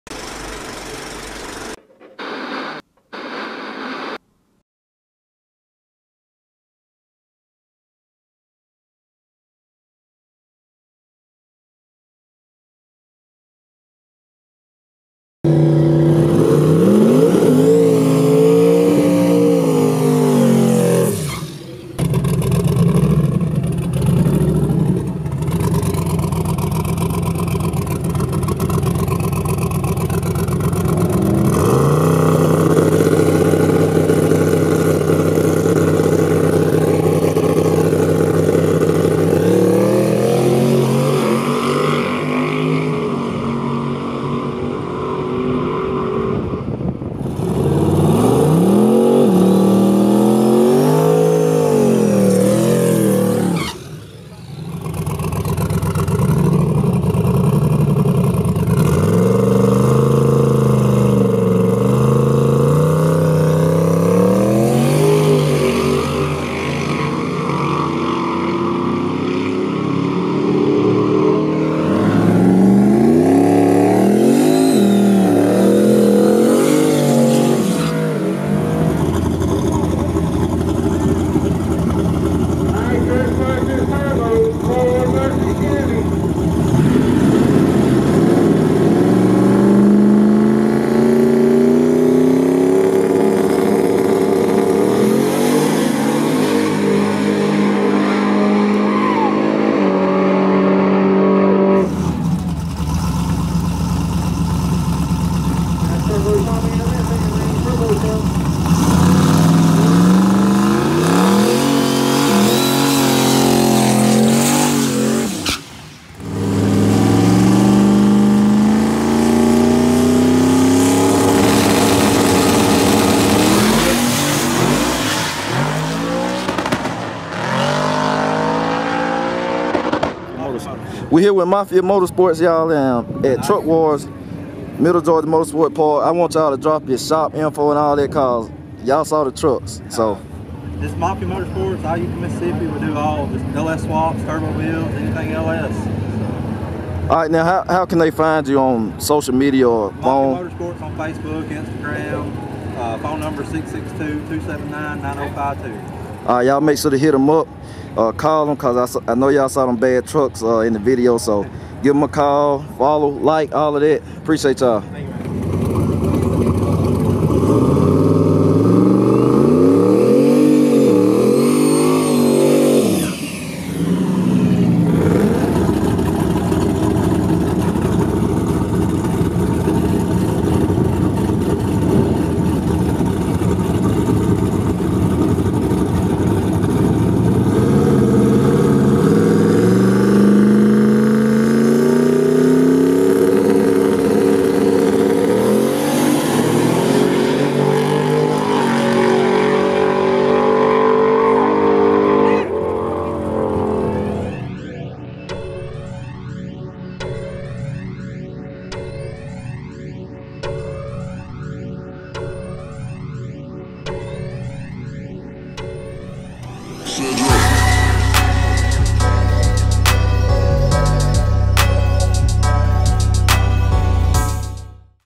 THESE LS TURBO TRUCKS OUT OF MAFIA MOTORSPORTS WERE DOING WORK!!
Some very nice trucks right here that were in the house at Truck Wars 3 at Middle Georgia Motorsports Park! Were getting down too!